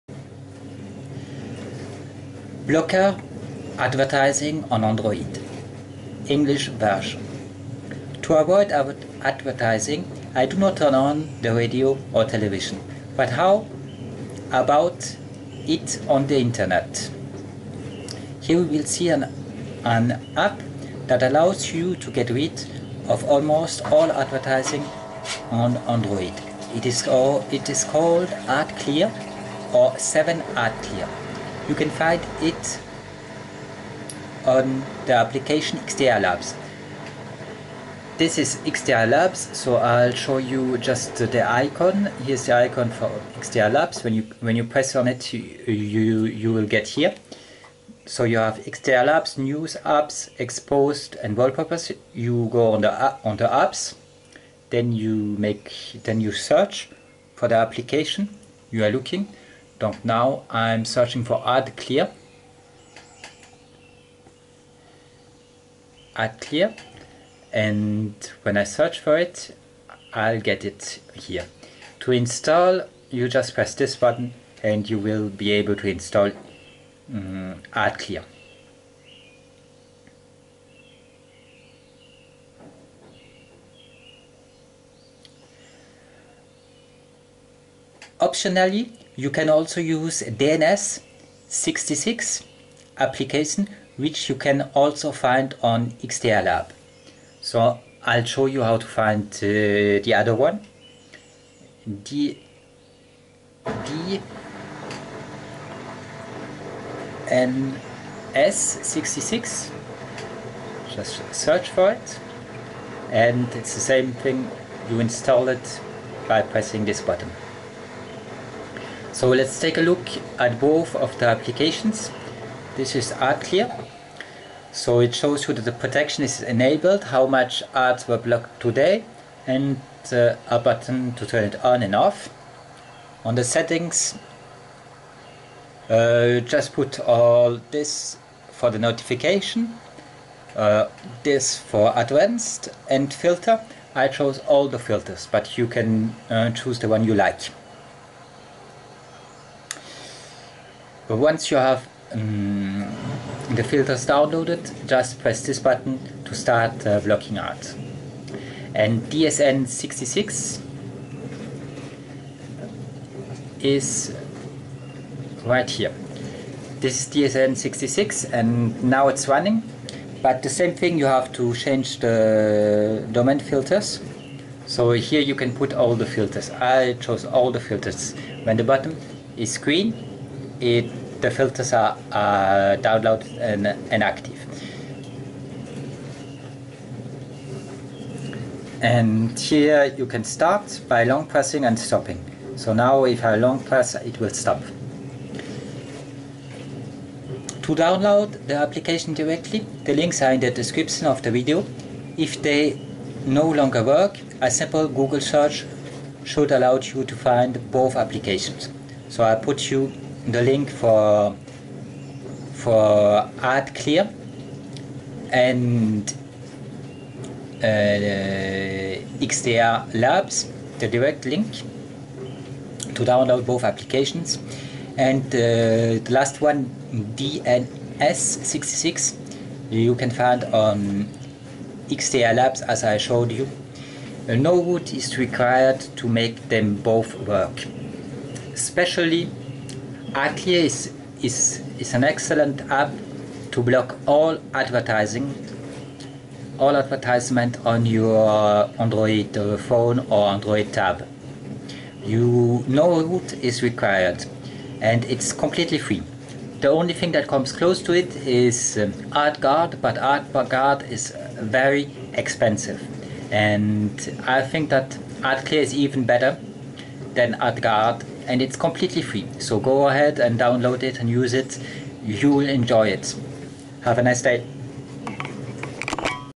Blocker advertising on Android. - version EN
To avoid advertising, I do not turn on the radio or television. "But how about it on the internet?" Here we will see an app that allows you to get rid of almost all advertising on Android. It is called "Adclear", or "seven Adclear", you can find on the application "XDA Labs".
Optionally, you can also use the "DNS66" application, which you can also find on "XDA Labs".
To download the application directly, the links are in the description of this video. If they no longer work, a simple Google search should allow you to find both applications.
3) "DNS66" on "XDA Labs".